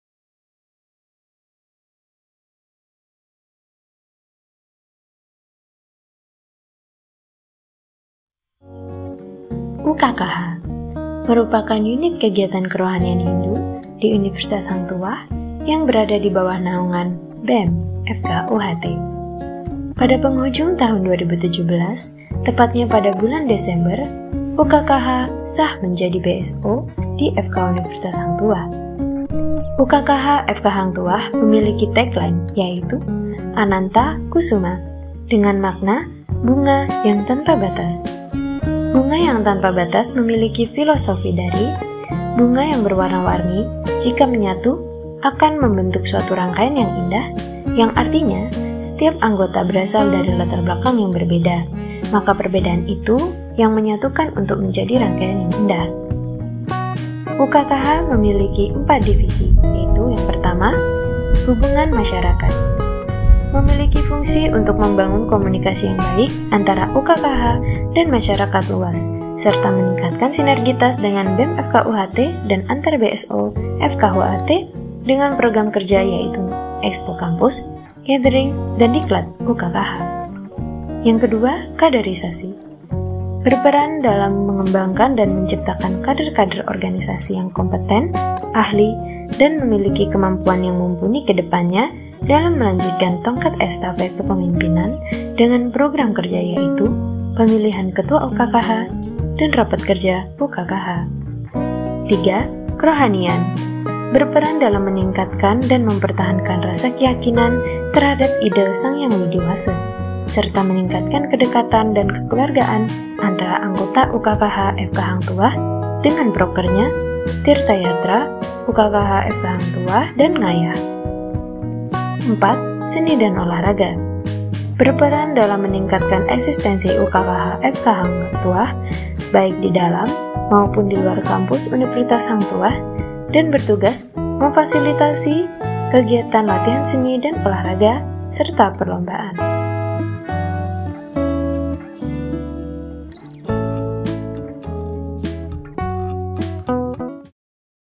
Video Profil - UKKH (Unit Kegiatan Kerohanian Hindu) FK UHT
Video profil sebagai tolak ukur pandangan terhadap organisasi tersebut dimana memiliki kelebihan,pengalaman, dan penghargaan yang di kelola dengan baik supaya dapat di lihat dan menarik minat masyarakat untuk menonton.

INFOKOM BEM proudly present EDTA 2020
“Enhance and Development of Applied Graphic Design Skills in 4.0 Era”